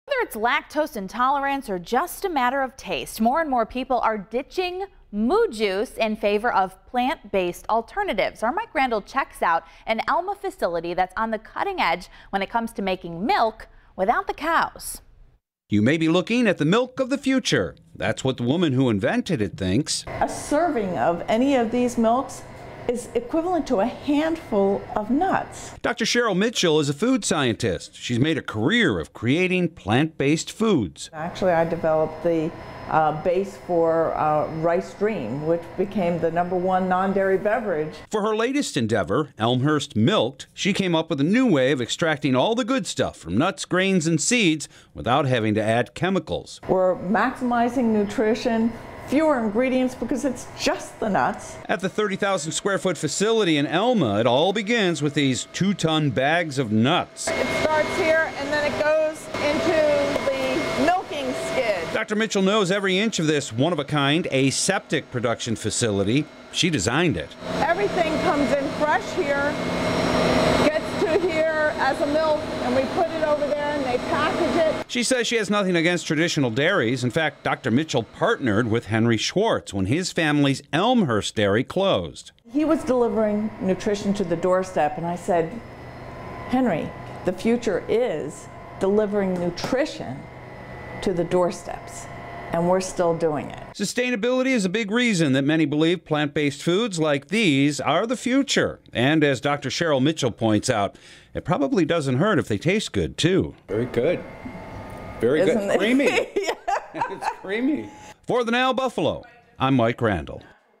She created the "milk of the future"
Dr. Cheryl Mitchell calls it the "milk of the future". She is a food scientist and the milk she is referring to is plant based, made from nuts, seeds and grains.  For her latest endeavor, a company called Elmhurst Milked, Dr. Mitchell came up with a new way of extracting all the good stuff from the nuts, turning it into milk without having to add chemicals. She says "we are maximizing nutrition because it's just the nuts and that's all we really need."  The thirty thousand square foot aseptic facility in Elma opened about a year ago. Dr, Mitchell knows every inch of the space because she designed it.  She partnered with with Henry Schwartz when his family's Elmhurst Dairy closed. Together they have created the new company Elmhurst Milked with the same purpose in mind as traditional dairies "delivering nutrition to the doorsteps."  You can find Elmhurst Milked at Wegmans and on line at the Elmhurst website